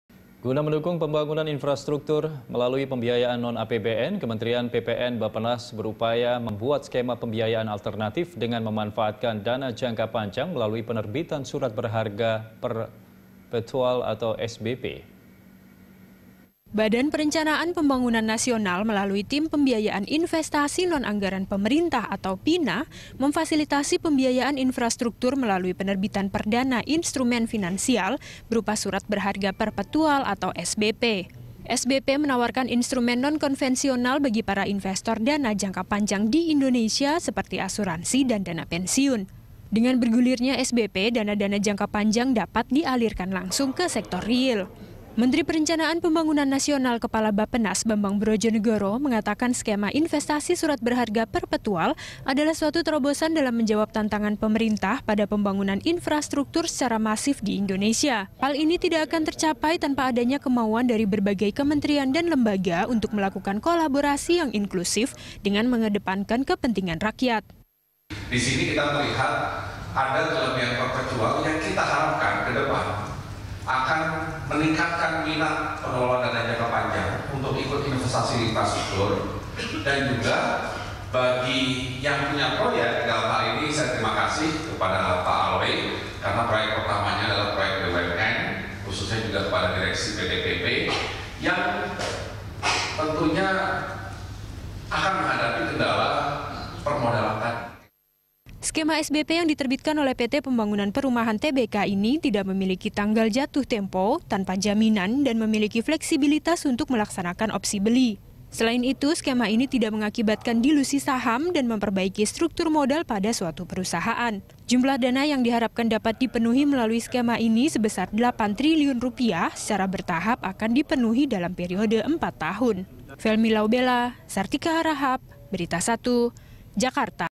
Bappenas Fasilitasi Penerbitan Surat Berharga Perpetual
Guna mendukung pembangunan infrastruktur melalui pembiayaan non-APBN, Kementerian PPN/Bappenas berupaya membuat skema pembiayaan alternatif dengan memanfaatkan dana jangka panjang melalui penerbitan surat berharga perpetual (SBP).

Facebook.com/BeritaSatuTV
Youtube.com/BeritaSatu